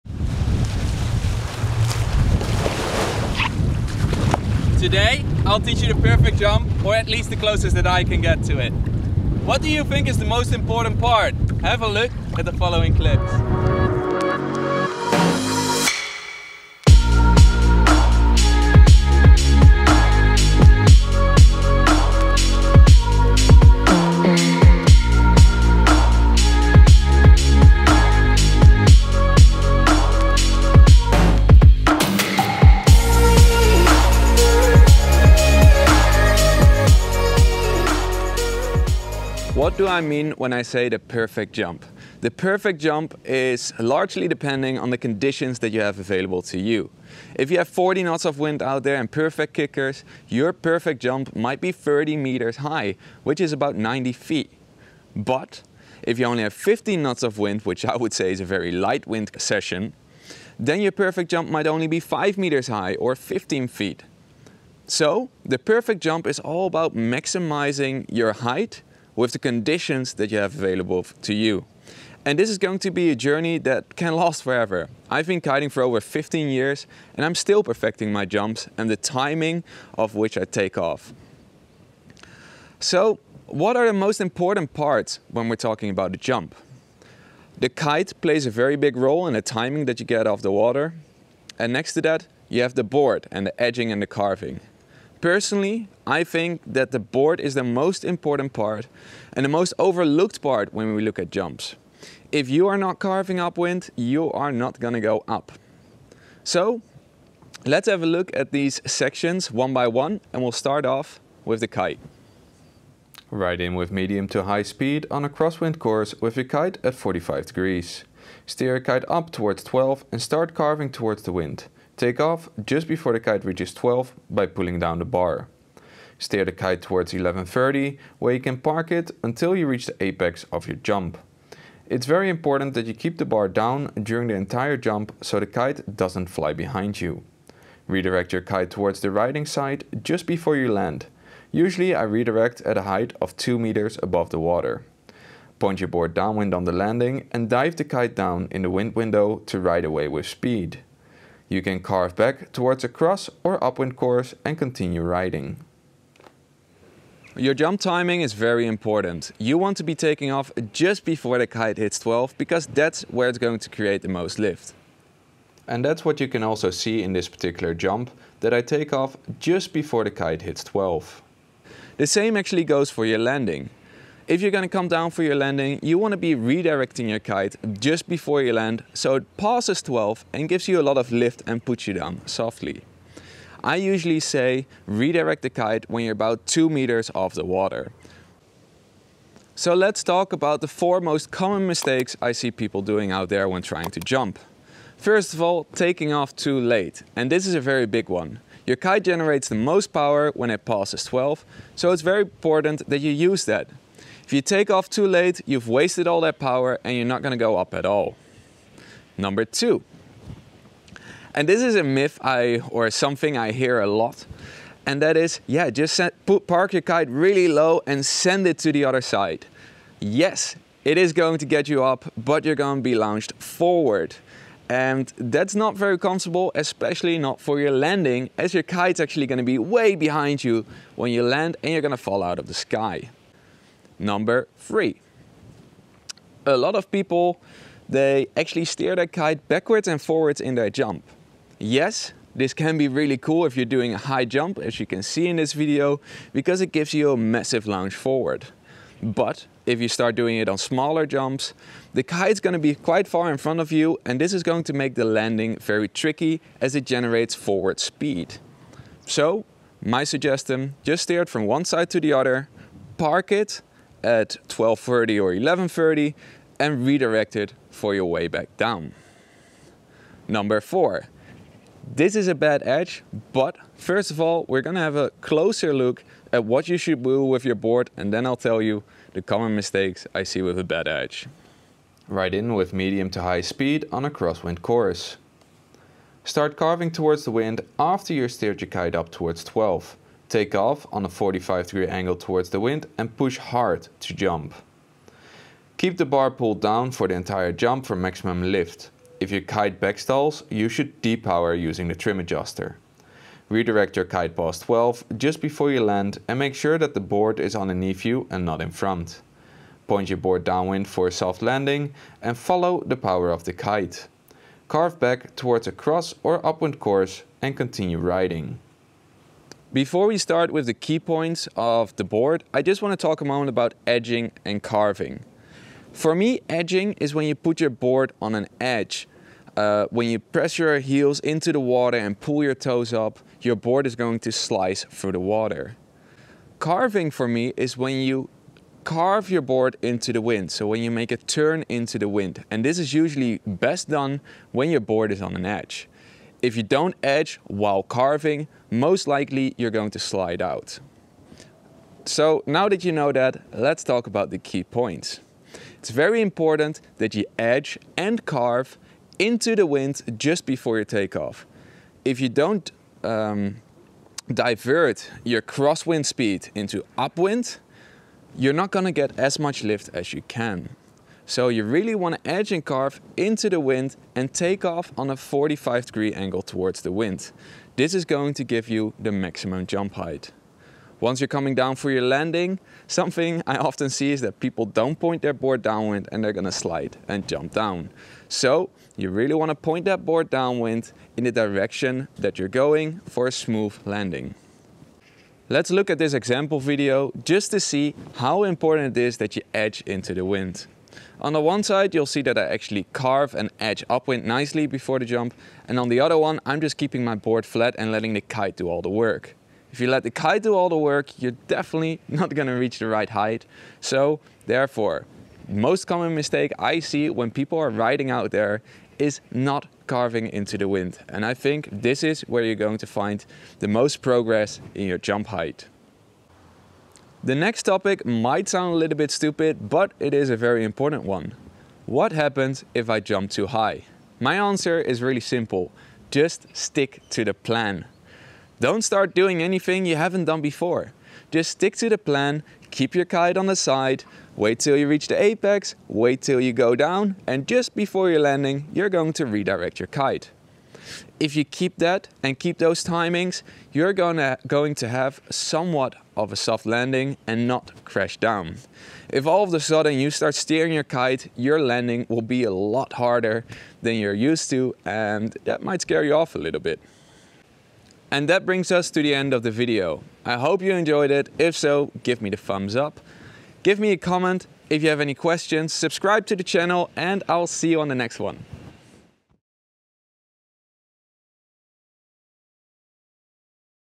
Boost higher with the PERFECT jump // Kiteboarding SA Masterclass
In this video I'll teach you how to jump high and maximize your jump height for big air. Want to beat your Woo record? This is the video to watch.

Kiteboarding / kitesurfing jump tutorial

00:39 What is the perfect jump?
01:52 Kite steering
03:09 Common mistakes kite steering
04:40 The board
07:05 Stick to the plan!

Filming / cover picture: José Denis-Robichaud

Do you want real time tips on the spot?

Video supported by:
CORE Kiteboarding